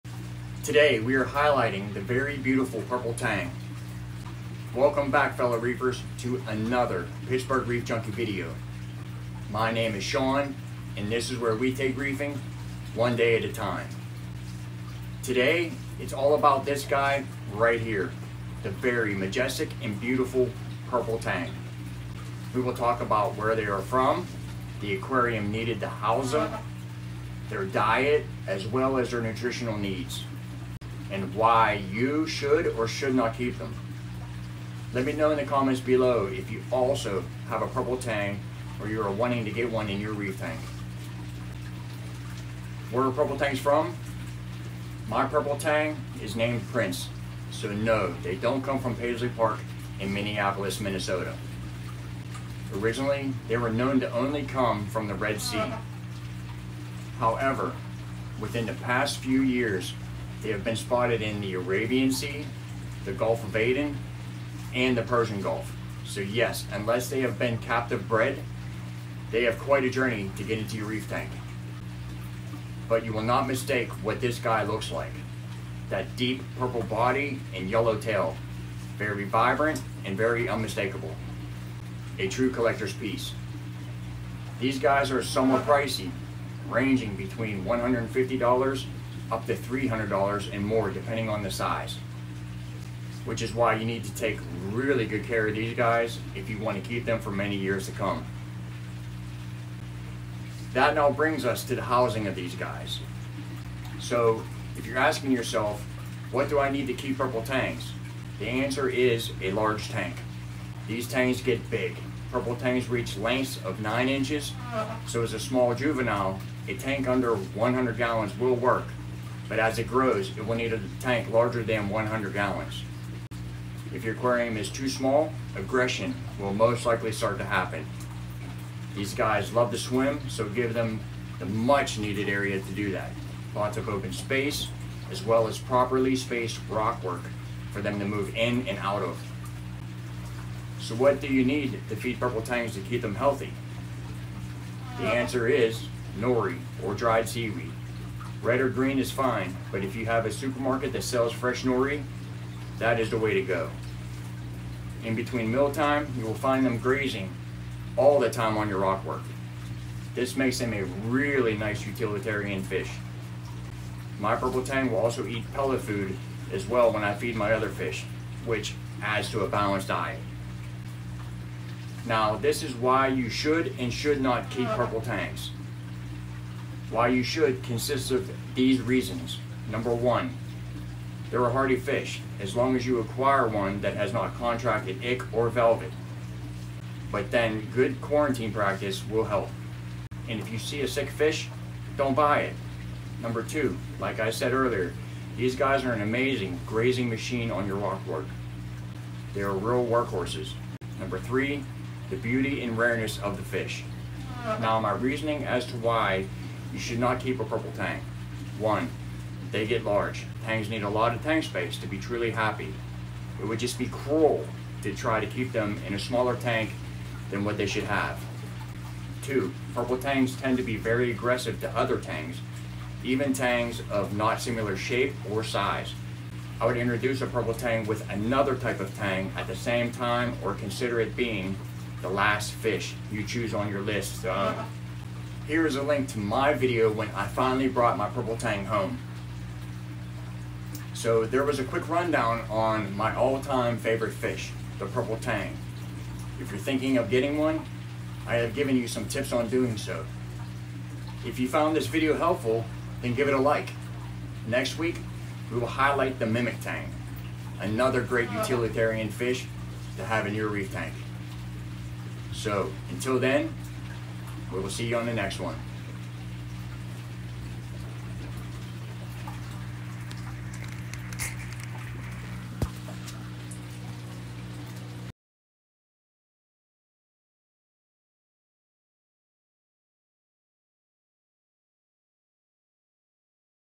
Purple Tang Care (Should You Keep One) How To Care for Purple Tangs
Red Nori Seaweed
Two Little Fishes Nori Clip

We talk about the Purple Tang. The Care of the Purple Tang. The aquarium size needed for the Purple Tang. What to feed your Purple Tang to keep them healthy. Inside the video we see our Purple Tang. The Purple tang is a very majestic and beautiful fish to own in any reef aquarium when the proper care requirements are met. Watch the video to help you decide if a Purple tang is right for you.